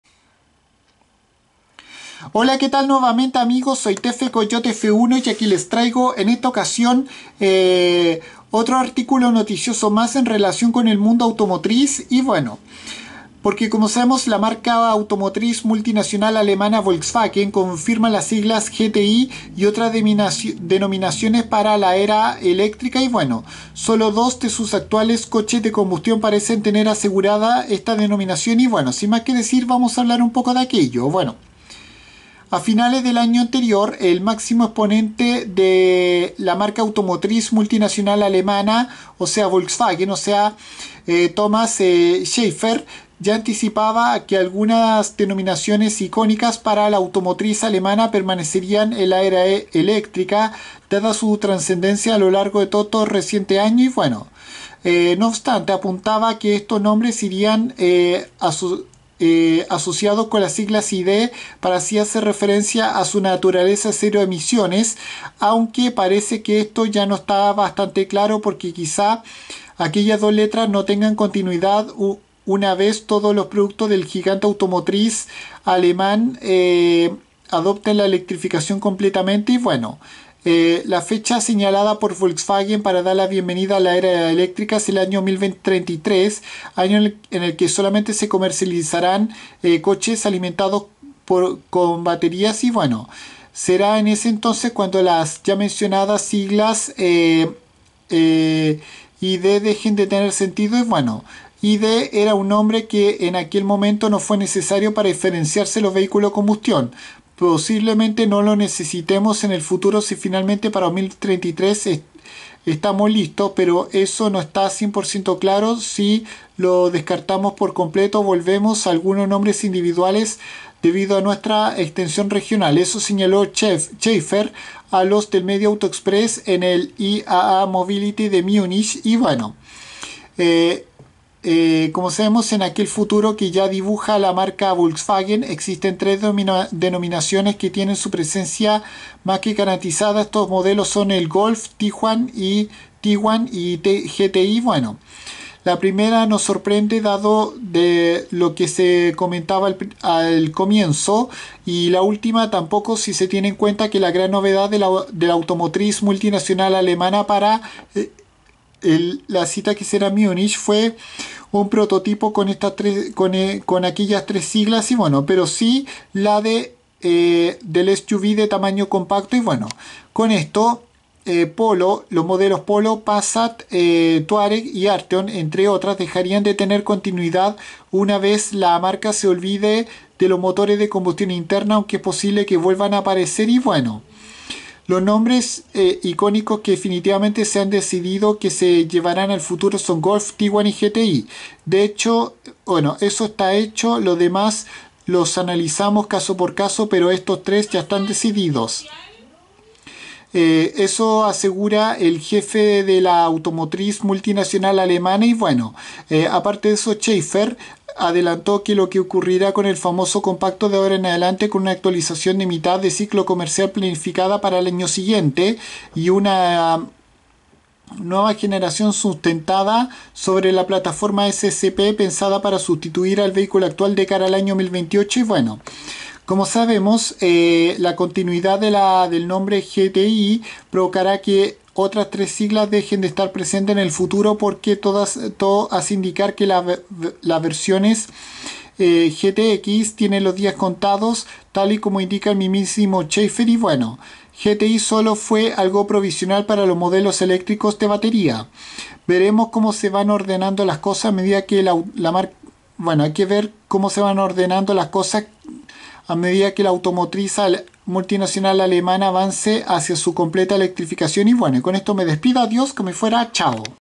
Volkswagen confirma que sus siglas GTI también estarán en la electrificación
Por el momento hay solo dos vehículos de combustión interna con esta denominación asegurada